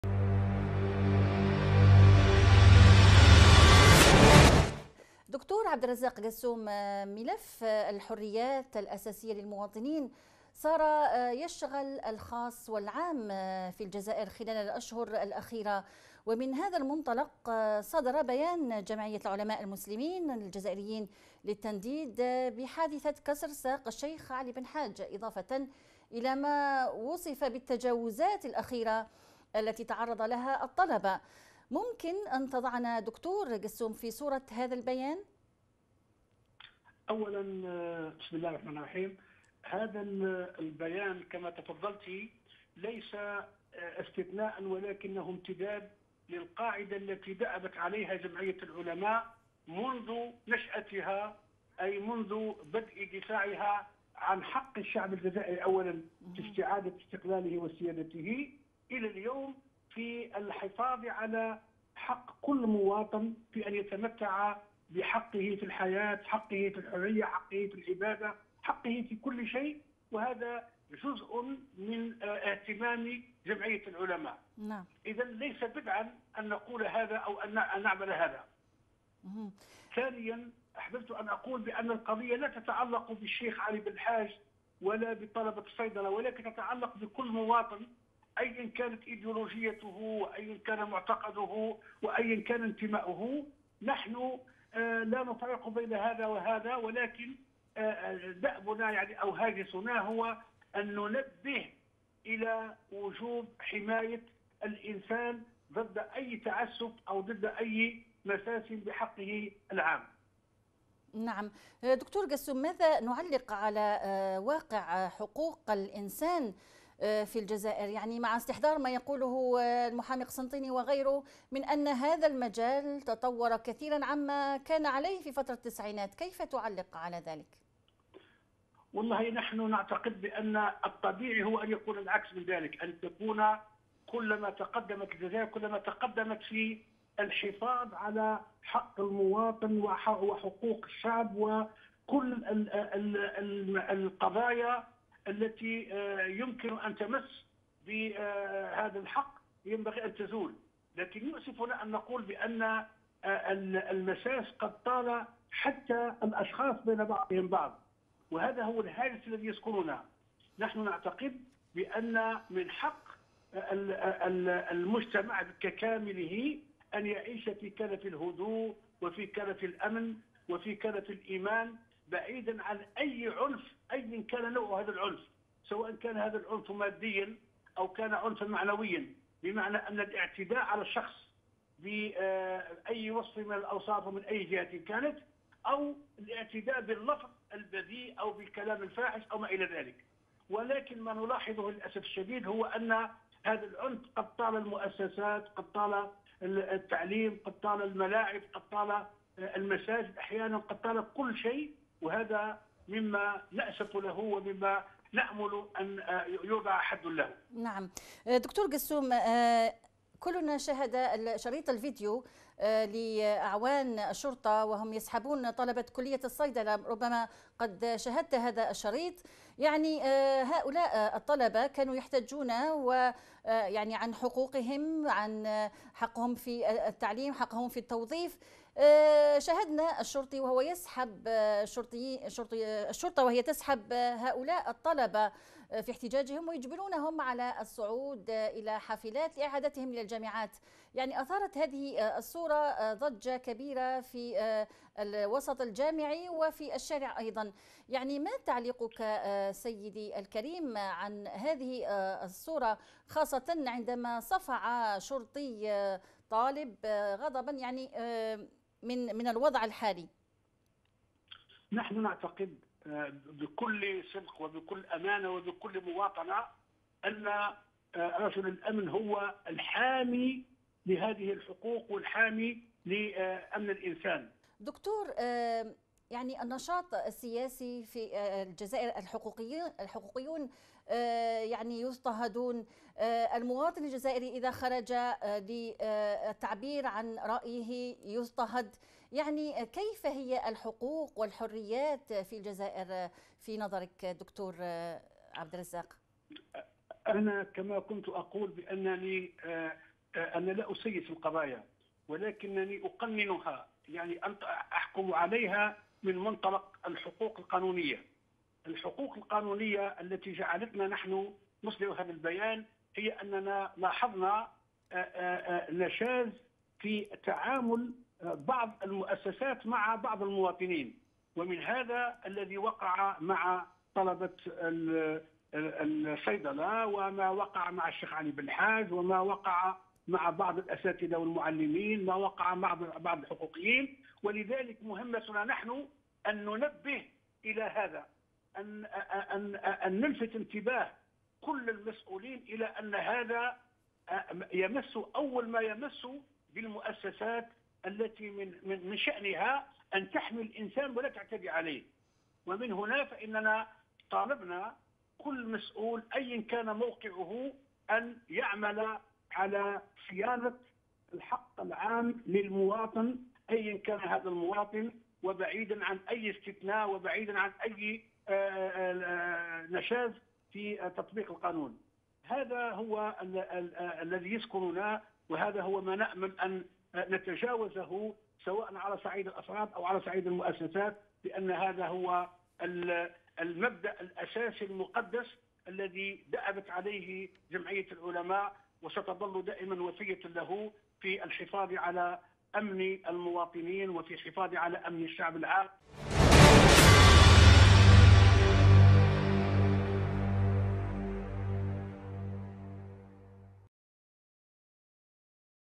د.عبد الرزاق قسوم رئيس جمعية العلماء المسلمين الجزائريين على قناة المغاربية
الجزائر.. المجتمع المدني يدق ناقوس خطر انتهاك الحريات.
مداخلة الدكتور عبد الرزاق قسوم رئيس جمعية العلماء المسلمين الجزائريين حول البيان الذي أصدرته الجمعية والذي يندد بالممارسات التي تمس الحريات العامة والخاصة للمواطن في الجزائر على غرار ما وقع مع الشيخ علي بالحاج.حيث اعتدى عليه رجال الأمن وكسروا رجله وما تعرض له طلبة الكليات وكلية الصيدلة من اعتداء وتنكيل اثر مظاهرات للمطالبة بحقوقهم التي يكفلها الدستور...

إن أمن البلد واستقراره نعمة يجب المحافظة عليها، ومسؤولية ذلك تقع على الجميع أفراداً وجماعات.
ولقد آلمنا ما وقع ضد مركز الأمن الحضري بقسطينة ،كما آلمتنا أوضاع مجتمعنا الجزائري المسلم، وما تفرزه، كل يوم، من تدهور أخلاقي، وتجاوزات على مختلف المستويات، كالعنف بجميع مظاهره.
وقد وصلت هذه الأوضاع المتردية ذروتها عندما وصل الأمر إلى التضييق على الحريات، والمساس بحقوق الإنسان، أفراد وجماعات.
وإن من النماذج السيئة، ما تعرض له مؤخراً المواطن، الأخ علي بلحاج، في وقت صلاة الجمعة، ومن قبله طلبة معهد الصيدلة. وسواء أكان هذا الاعتداء عن قصد أو دون قصد، وسواء تعلق الأمر بالأخ علي بلحاج، أو أي مواطن آخر، أيا كانت إيديولوجيته، فإن مثل هذا يتنافى مع قوانين الجمهورية، وما تنشده جمعية العلماء المسلمين الجزائريين، من الحفاظ على كرامة الإنسان، وحريته في جزائر العزة والكرامة.
لذا تعرب جمعية العلماء المسلمين الجزائريين، عن بالغ تأسفها لما آلت إليه الأمور في هذا المجال، وتهيب بالمسؤولين، كل في ميدان تخصصه، أن يعملوا على صيانة كرامة المواطن، وعدم إهدار حقوقه الأساسية، وفي مقدمتها حرية الرأي المسؤولة.
كما نأمل أن لا تتكرر مثل هذه الحوادث، التي طالت دعاة، وصحفيين، ومعلمين، ونقابيين من قبل، فإن ذلك من شأنه تقديم صورة غير مشرفة لجزائرنا التي نأمل أن تنعم بالاستقرار والأمن والأمان الذي هي في أمس الحاجة إليه. إن ضمان السير المتزن، هو الكفيل ببناء المجتمع الإسلامي الأفضل، والله في عون المواطن مادام المواطن في عون أخيه.